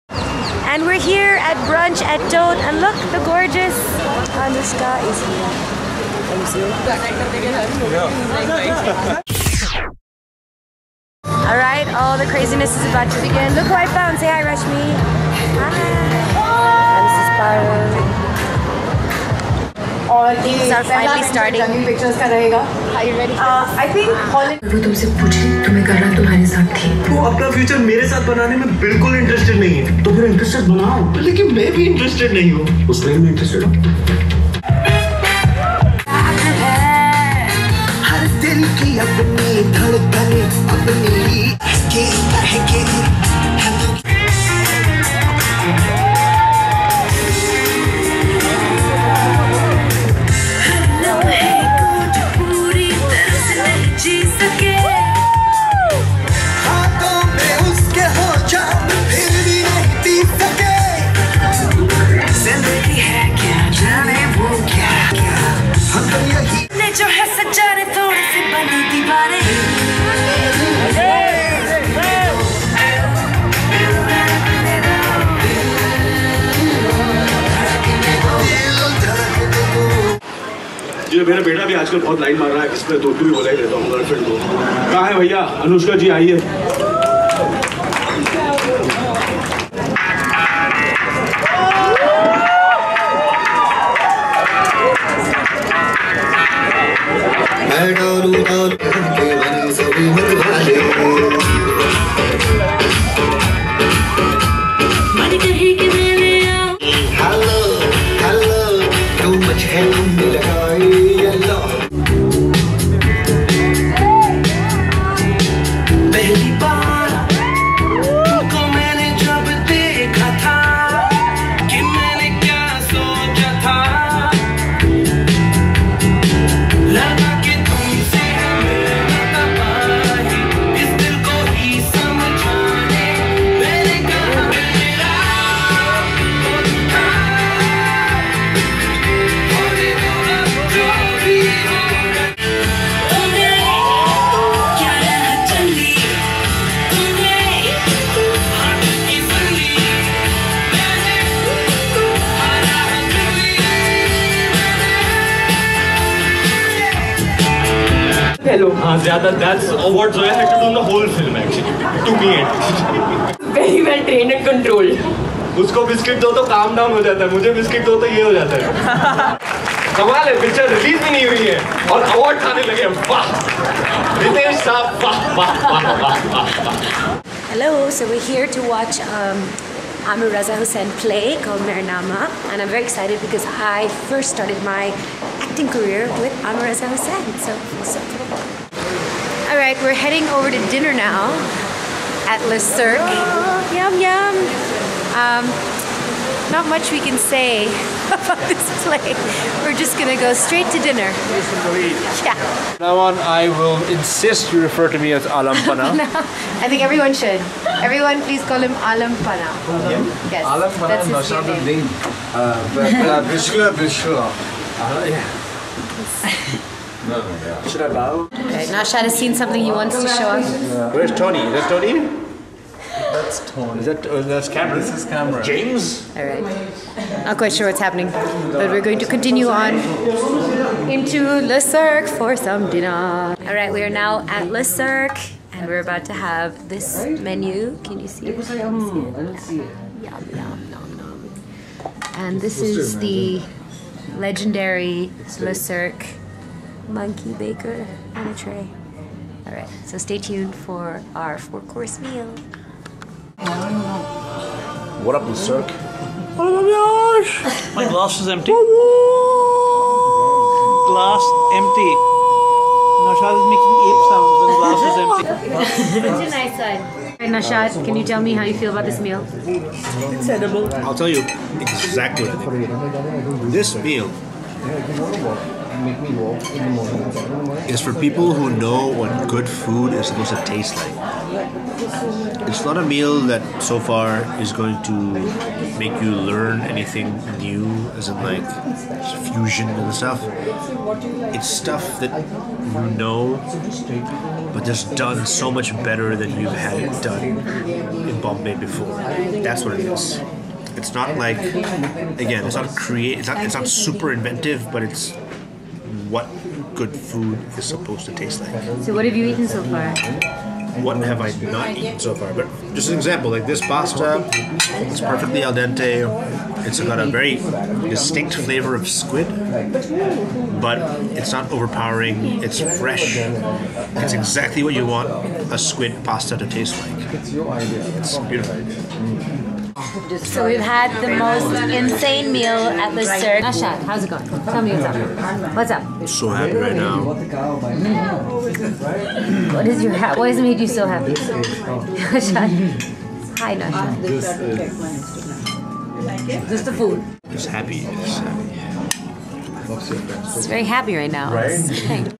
Want To See How Ranveer Singh Behaves At Parties? Vlog #3
I dropped in on the Dil Dhadakne Do brunch and later we had dinner at Le Cirque! Catch all the action, raw and unplugged on my new vlog and all the deets on MissMalini.com :)